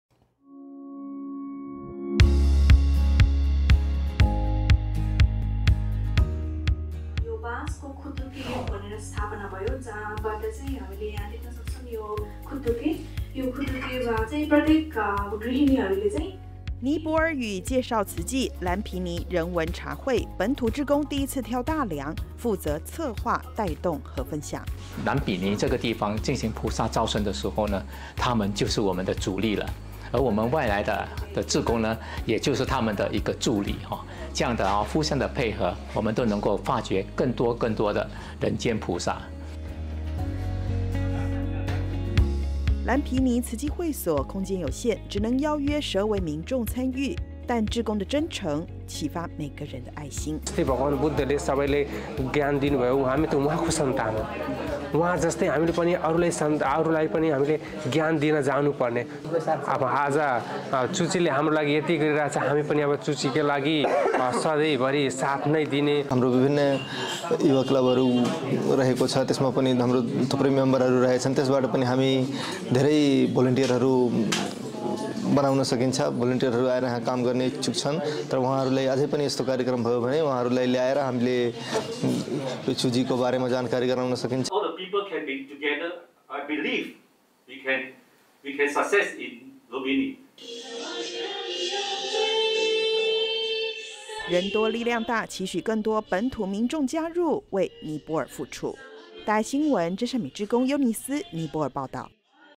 接引尼泊爾在地志工 佛陀故鄉造淨土
慈濟志工在尼泊爾藍毗尼舉辦茶會，介紹慈濟，希望有更多當地志工加入，一起為佛陀故鄉付出，翻轉更多人的命運。

尼泊爾志工：「慈善志業從竹筒歲月開始，由幾位家庭主婦發起。」

尼泊爾語介紹慈濟，藍毗尼人文茶會，本土志工第一次挑大梁，負責策劃，帶動與分享。

新加坡慈濟志工 吳南凱：「藍毗尼這個地方，進行菩薩招生的時候，他們(本土志工)就是我們的主力，而我們外來的志工，也就是他們的一個助力，這樣互相地配合，我們都能夠發掘更多更多的人間菩薩。」

藍毗尼慈濟會所空間有限，只能邀約12位民眾參與，但是志工的真誠，啟發每個人的愛心。

民眾 拉古瑪：「我們是佛陀的後人，應該像佛陀一樣，分享智慧(給每個人)，慈濟幫助我們很多，現在我們也要協助慈濟。」

民眾 拉卡斯：「我們青年團有很多會員，可以從這裡招募志工，他們有意願與慈濟合作，如果慈濟有這類活動，我們可以邀約他們參加。」

人多力量大，期許更多本土民眾加入，為尼泊爾付出。

真善美志工 攸尼斯 尼泊爾報導